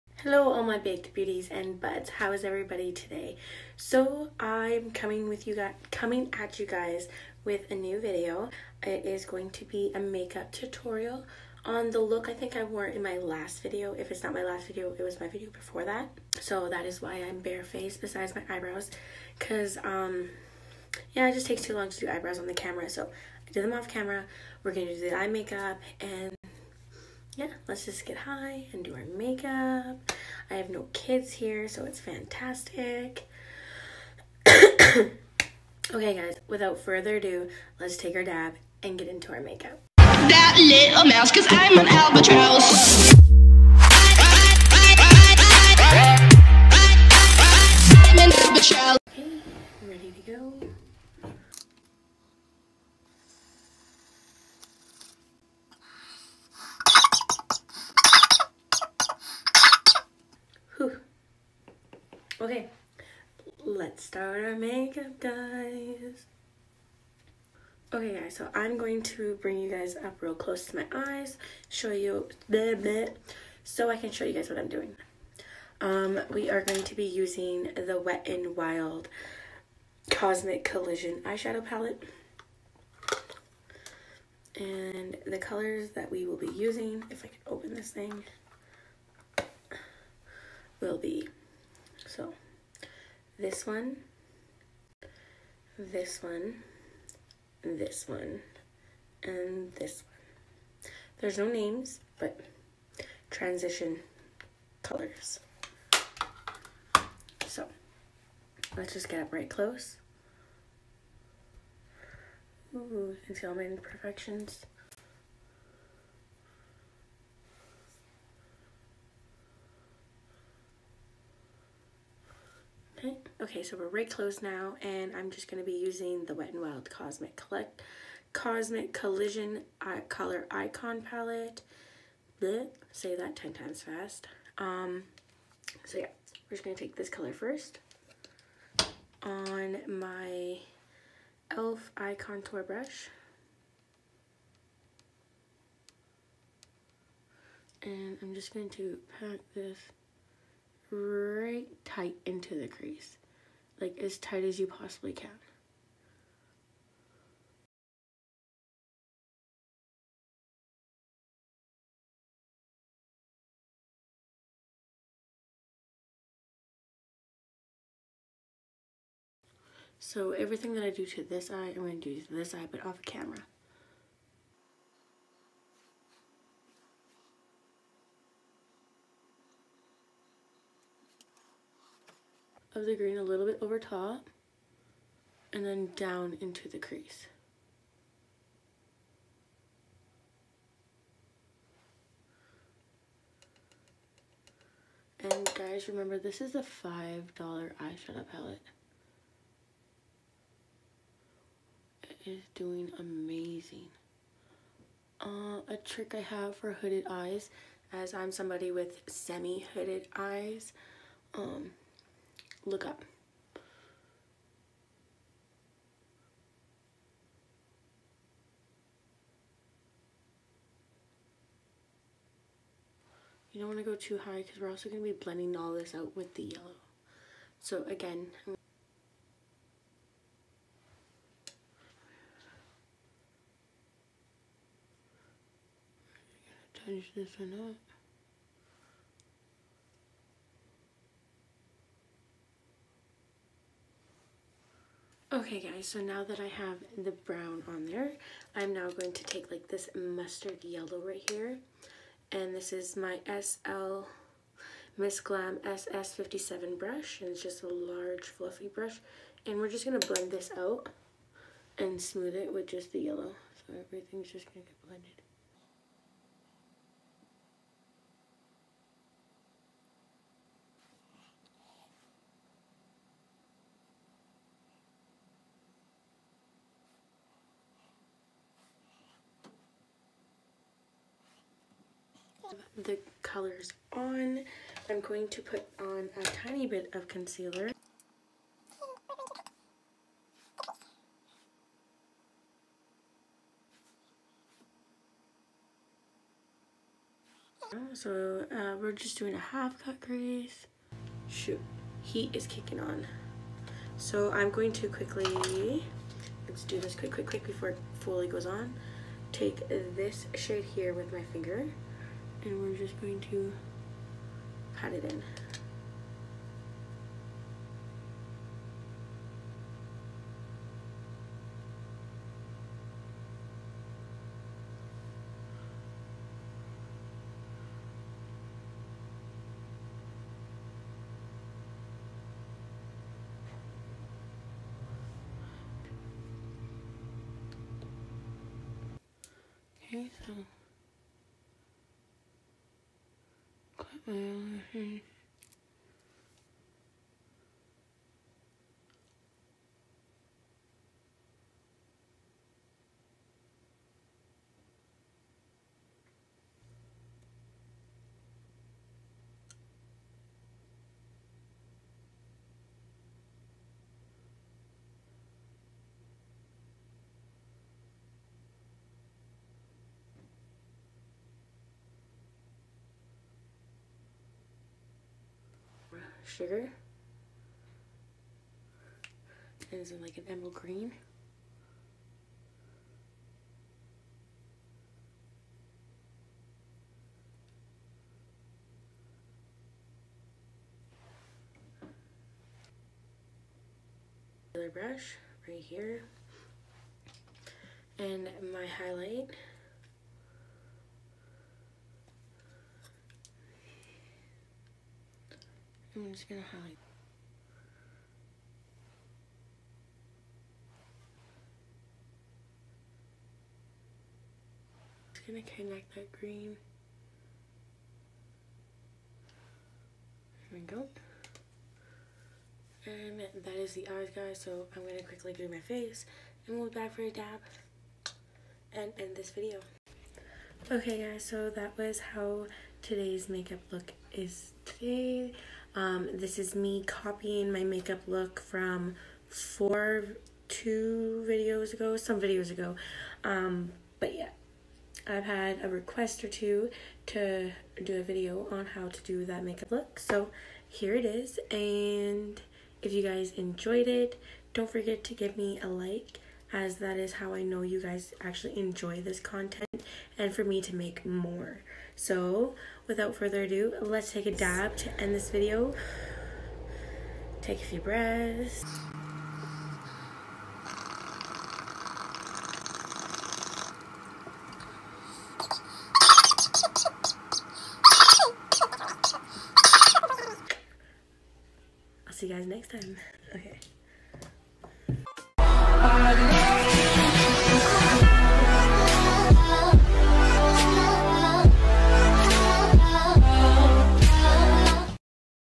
Golden and Green Eye look with Wet&Wild
Here’s a video of my eyeshadow look I have been rocking for a few days now and I’m sooo in love with it. I have had a few people in RL ask me how to do it as well as Instagram and here so I figured I would give them  what they wanted and hopefully it’s what you guys will want too.

😝💋 Kimberley💋😘